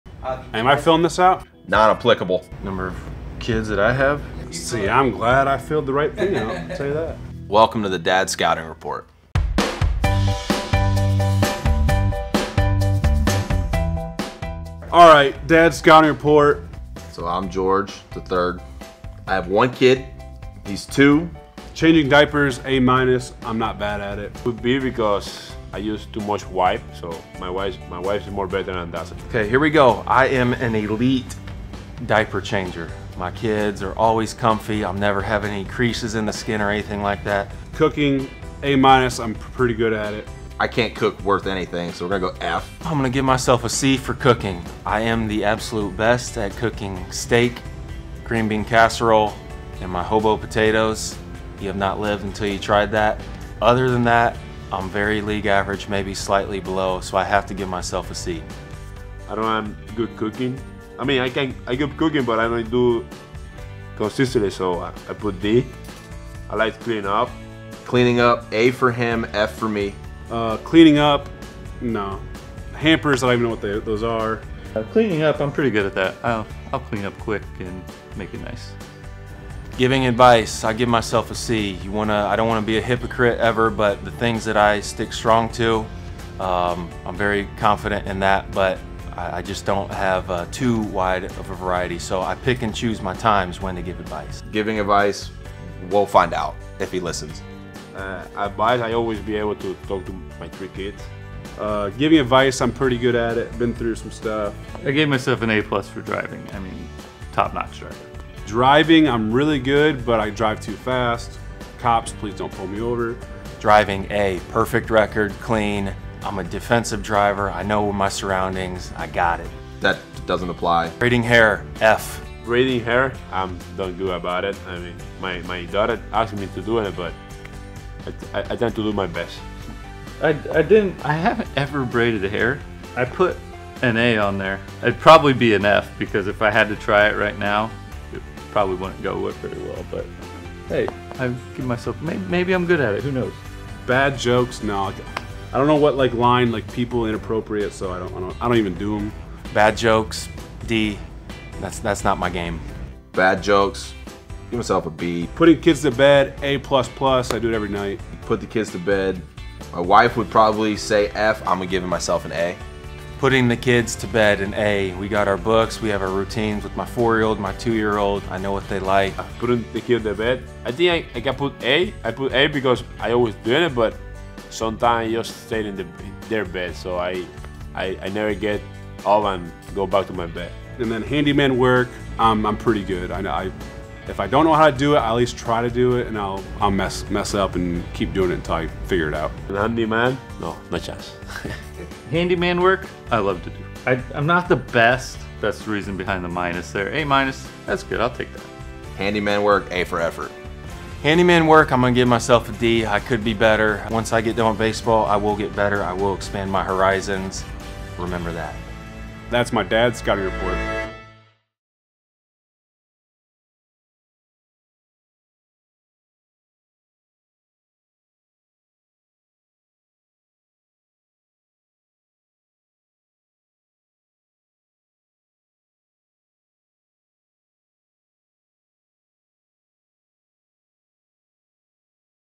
We asked these Blue Jays to give us a scouting report on themselves...as Dads!
For FathersDay, we asked George Springer, Chris Bassitt, Kevin Kiermaier, José Berríos and Trevor Richards to grade their dad skills for a scouting report!